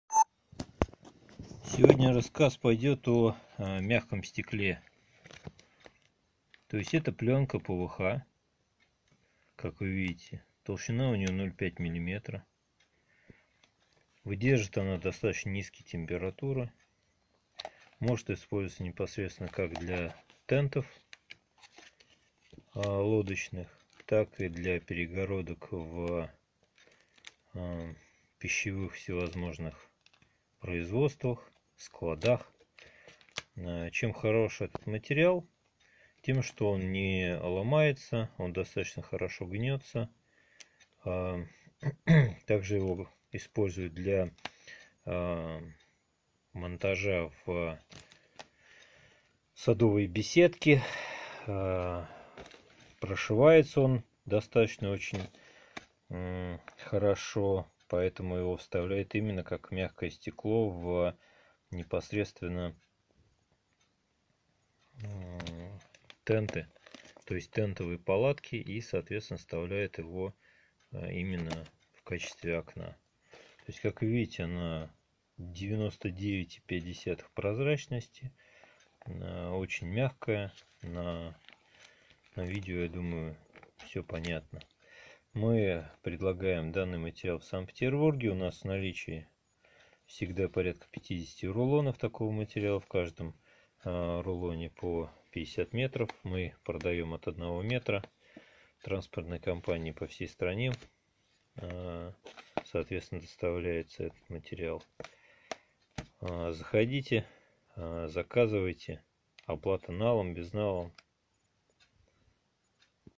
Мягкое стекло тентовая ткань
Мягкое стекло тентовая ткань Мягкое стекло для тентов, окон, беседок и другого, продажа в отрез от 1 м.п. со склада в Санкт-Петербурге. Отлично сваривается СВЧ с минимальным слипанием слоев. Морозостойкостью до -45С, толщиной от 0,2 до 0,5 мм и жесткостью 42 PHR. Используется для изготовления различной упаковки под косметику (косметички), сувениры, игрушки, постельные принадлежности, окон для пологов палаток тентов (мягкое стекло), теплиц и т.д.